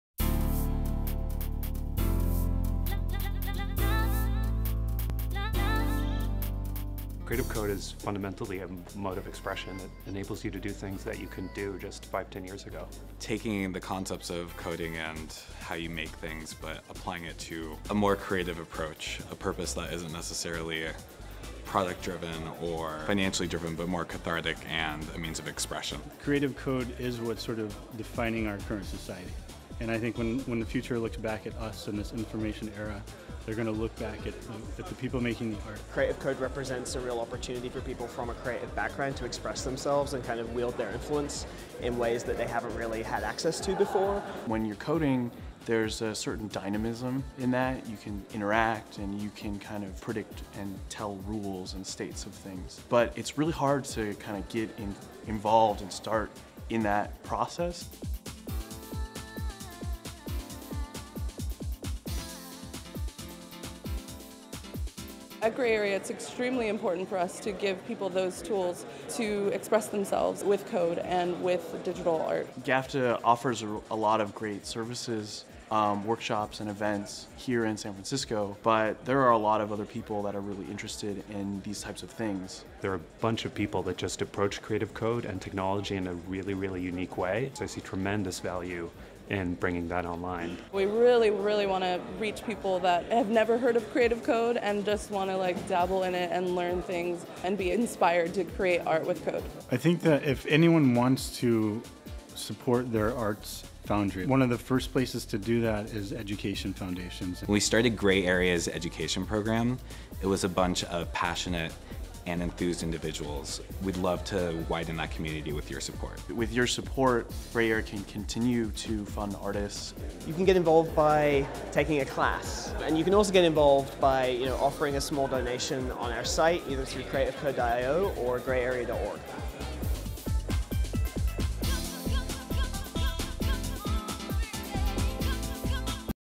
Teaching Code to Create Art: Help us Share our Education Program Online, Creativecode.io
Since 2008 we have been facilitating Creative Coding and Physical Computing Classes. We have held over 50 classes and reached over 500 students in San Francisco. We want to expand on this community and take our classes online by developing a new platform called creativecode.io

Attend the Techno Casino Fundraiser on June 8th at The Regency Ballroom to support the expansion of the Gray Area Education program